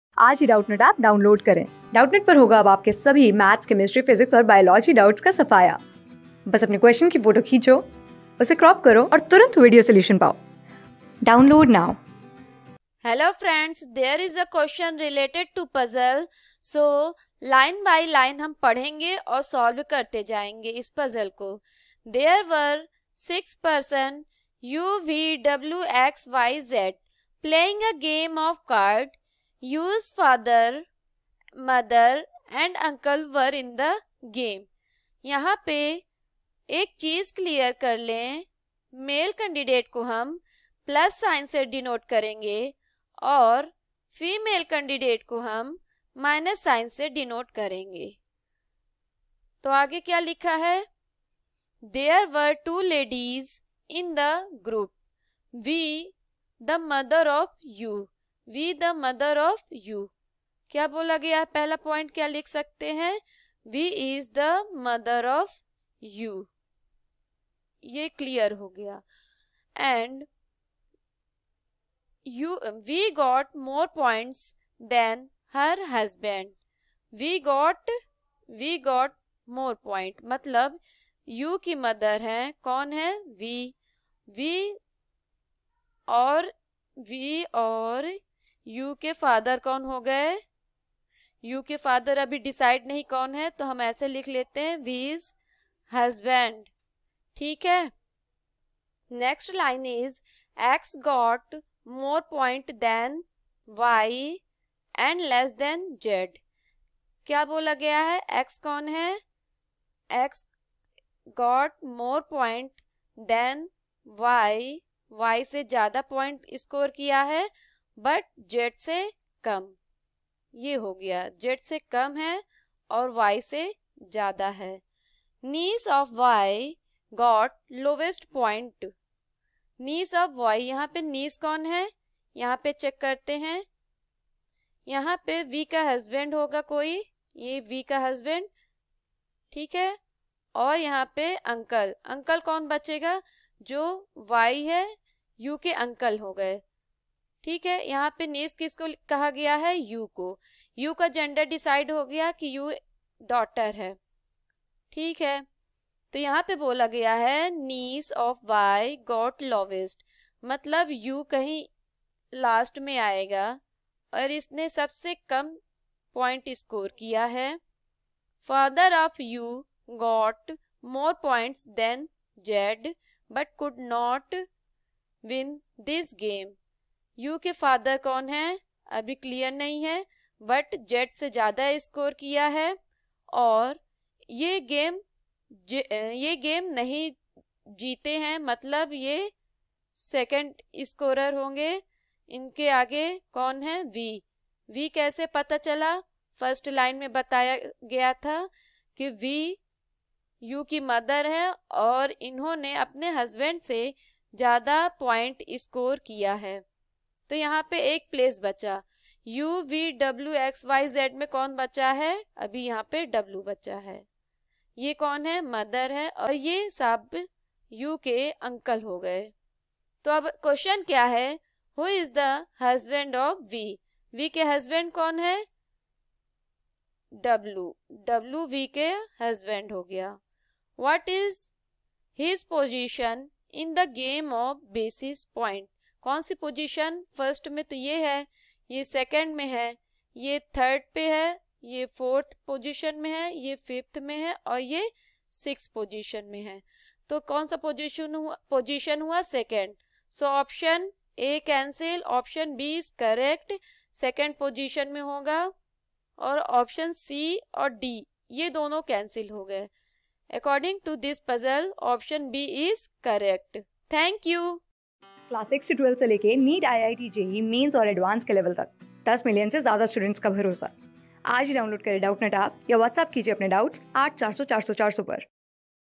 There were six persons U,V,W,X,Y,Zplaying a game of cards. U\'s father, mother and uncle werein ...
There were six persons U,V,W,X,Y,Zplaying a game of cards. U\'s father, mother and uncle werein the group.There were two ladies in the gsoup. \'V\' the mother of \'U\' got more points than her husband. \'X\' got more points man \'Y but less than \'Z . Niece of \'Y\' got lowest points Father of \' U\' got more points than \'Z\' but could not win die game. Who is the husband of \'V\' and what was his position in the game on the basis of points ?
Class: 14
Subject: REASONING
Chapter: PRACTICE SET 10
Board:GOVT

👉 Have Any Query? Ask Us.